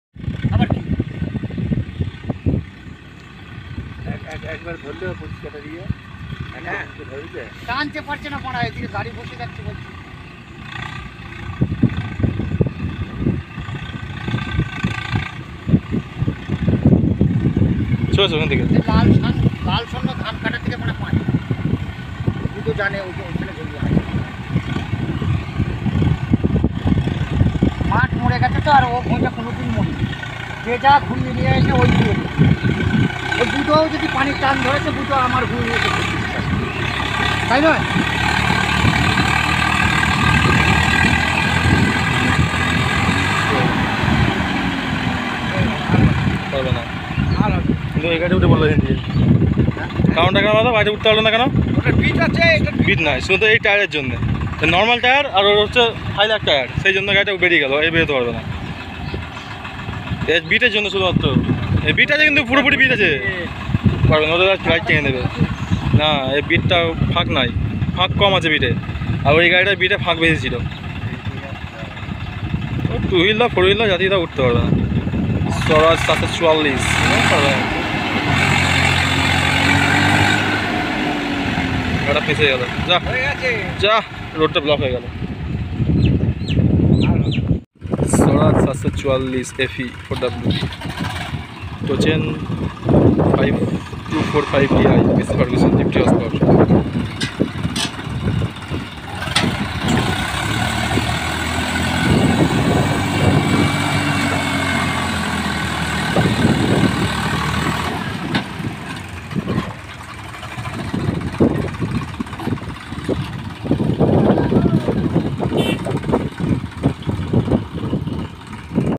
Swaraj 744FE 4wd Stuck in MUD Rescue by Massey Ferguson 5245 4wd
Swaraj 744FE 4wd Stuck in MUD Rescue by Massey Ferguson 5245 4wd || LM tractor vlog

                  Highlight video

tyre- normal